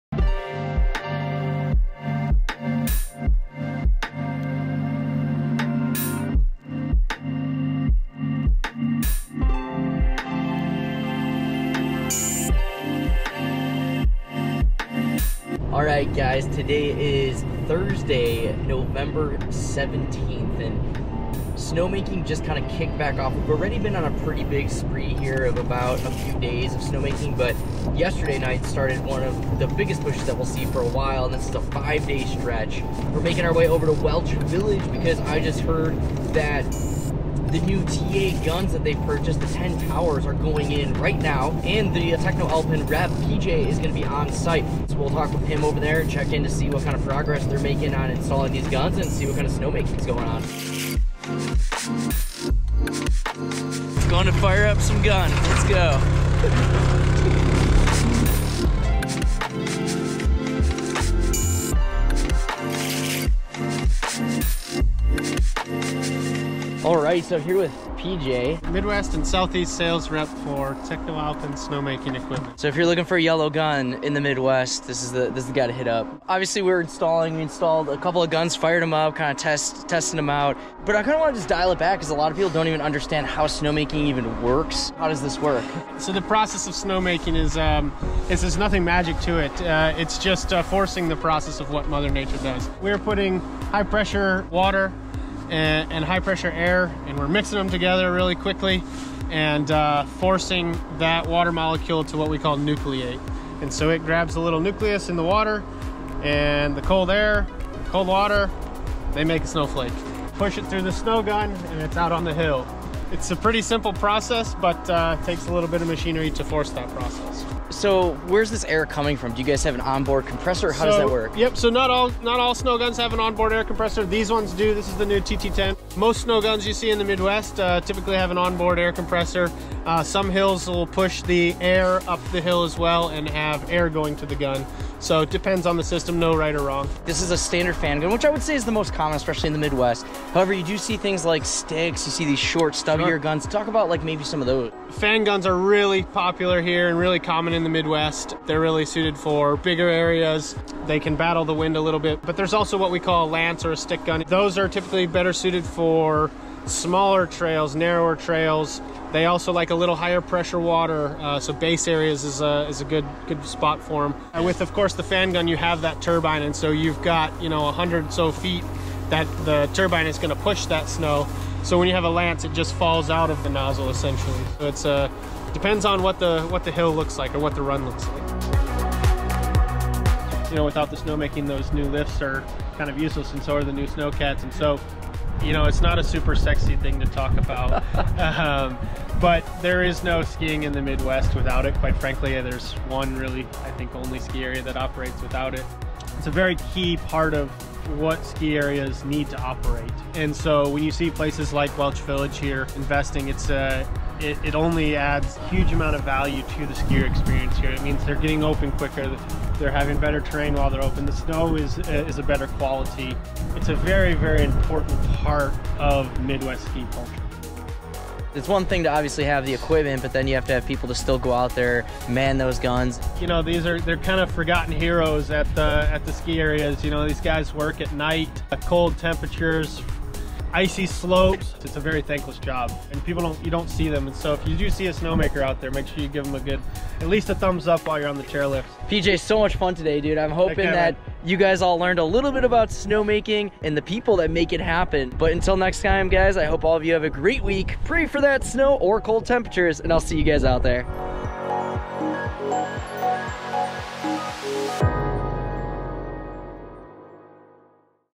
Installing New Snow Guns At Welch Village!
Join me as I travel to Welch to help PJ from TechnoAlpin and the Welch crew install their brand-new TR10s. We chat about what it means to make snow and what snowmaking means to the Midwest ski/board scene.